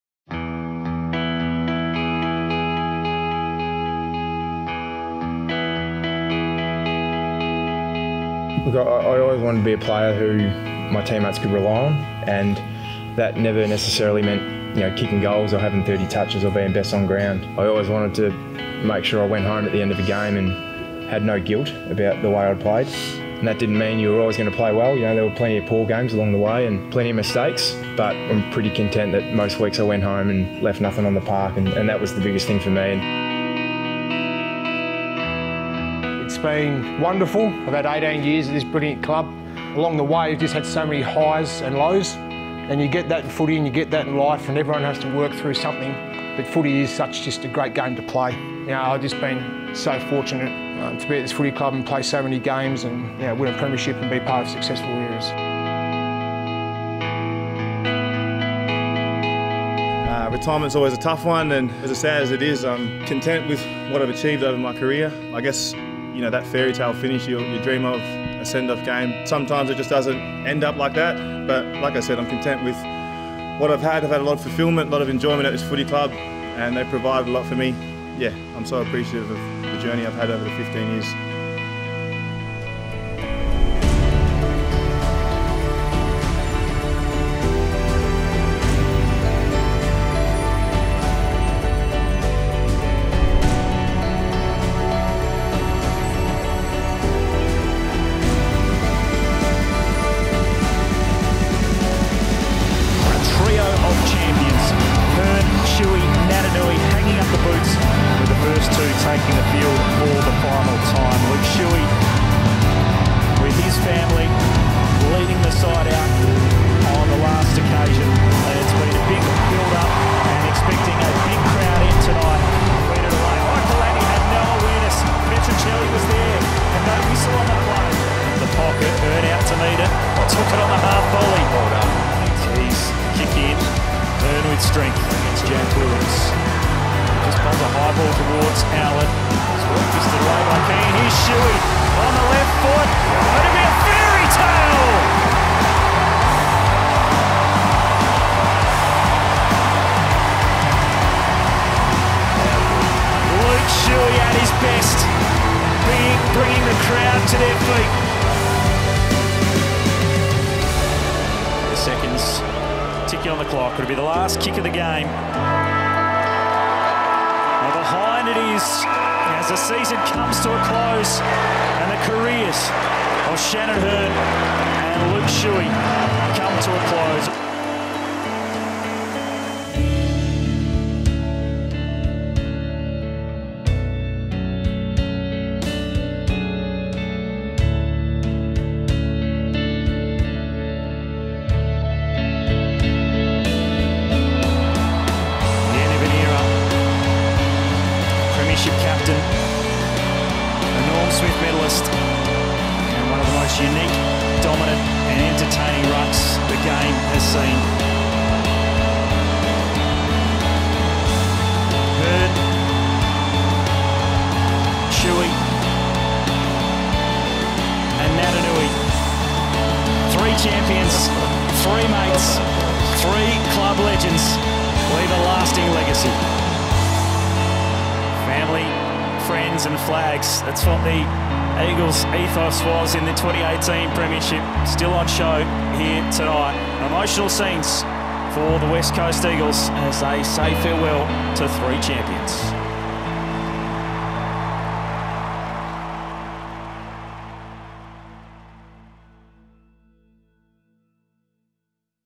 Farewell to our heroes
Take a look back at our final game of 2023 as we say goodbye to three West Coast legends